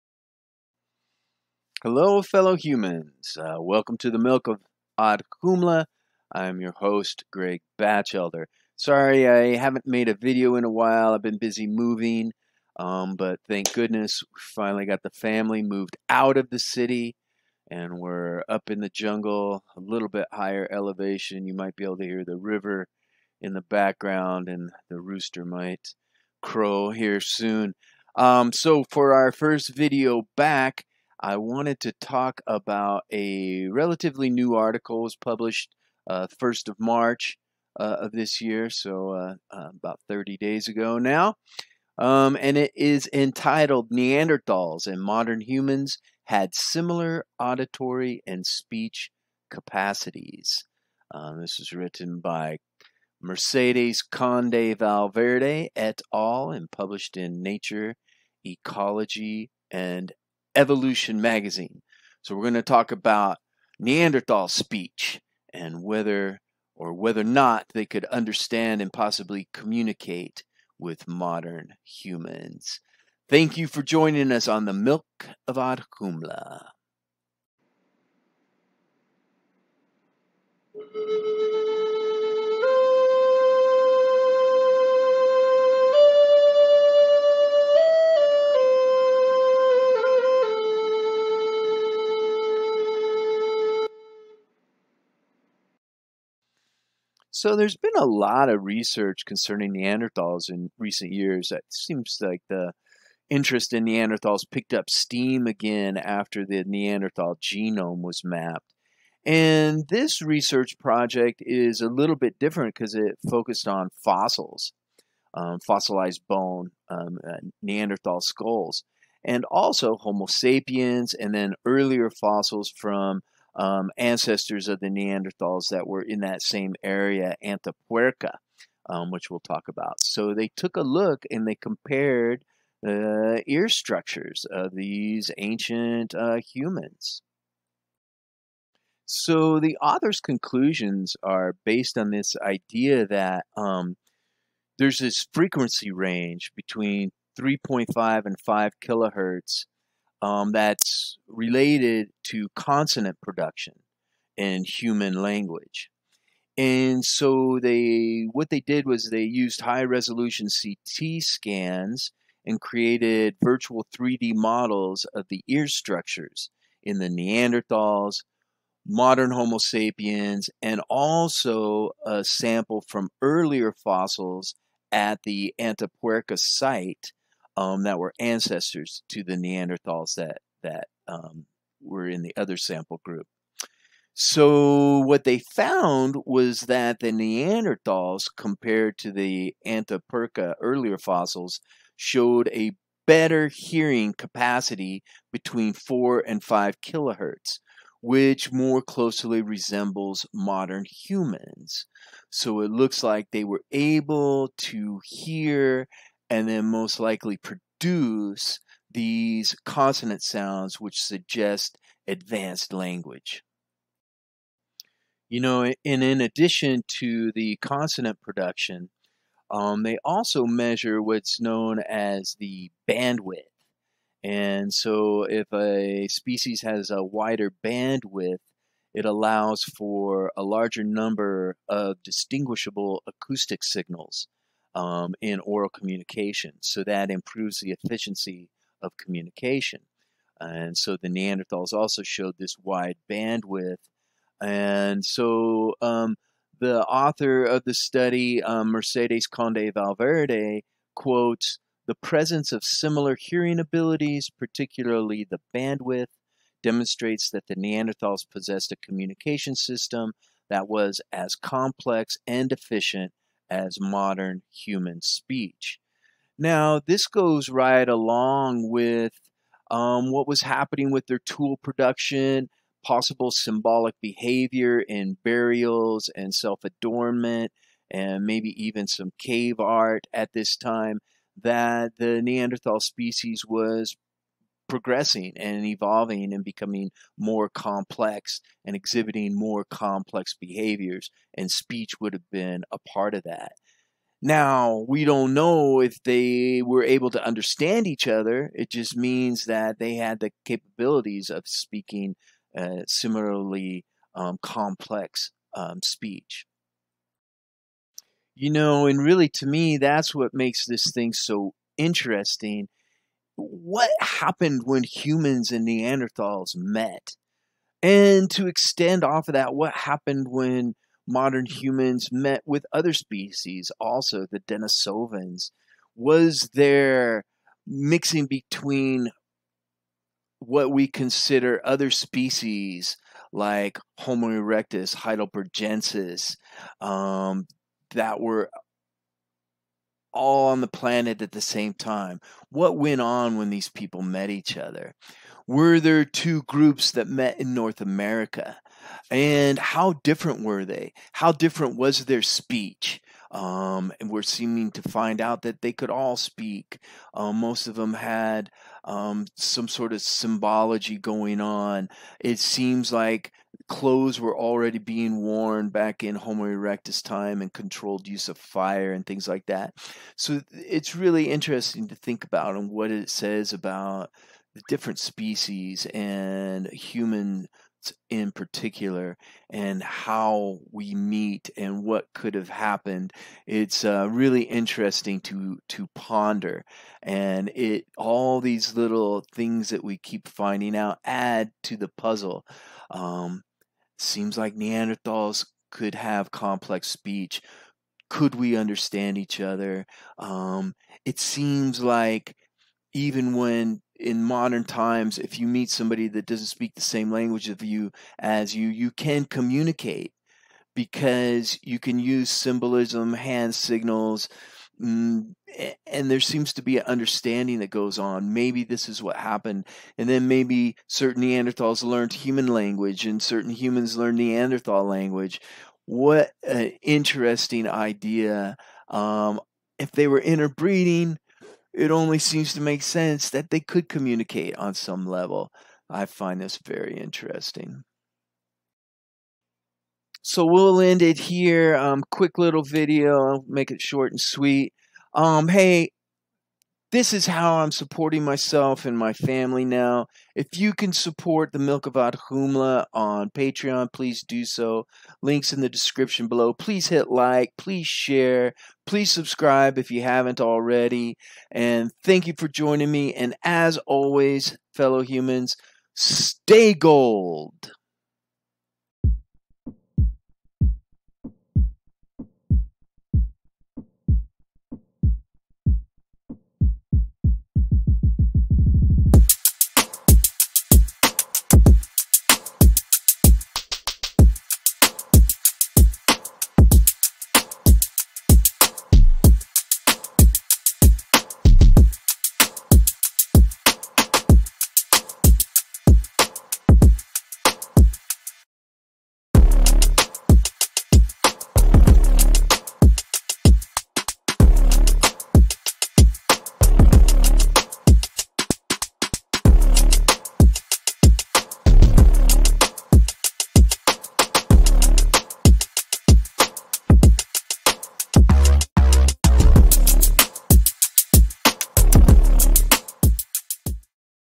Neanderthal Language: Perception and Production of Human Speech.
Reference: “Neandertals and modern humans had similar auditory and speech capacities” by Mercedes Conde-Valverde, Ignacio Martínez, Rolf M. Quam, Manuel Rosa, Alex D. Velez, Carlos Lorenzo, Pilar Jarabo, José María Bermúdez de Castro, Eudald Carbonell and Juan Luis Arsuaga, 1 March 2021, Nature Ecology and Evolution.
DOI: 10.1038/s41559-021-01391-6

As always, thank you for joining me, and Stay Gold.

7-3D model and virtual reconstruction of the ear in a modern human (left) and the Amud 1 Neandertal (right). Credit: Mercedes Conde-Valverde
8-Reconstructed hearing patterns in modern humans, Neandertals and the Sima de los Huesos based on their ear anatomy. Compared with their ancestors from the Sima de los Huesos, the Neandertals more closely resemble modern humans in showing a heightened sensitivity between 3.5-5 kHz, a frequency range that contains acoustic information related to consonant production in human spoken language. Credit: Mercedes Conde-Valverde
9-By Jaroslav A. Polák from Brno, Czech Republic - Neanderthal Family (detail of diorama), CC0,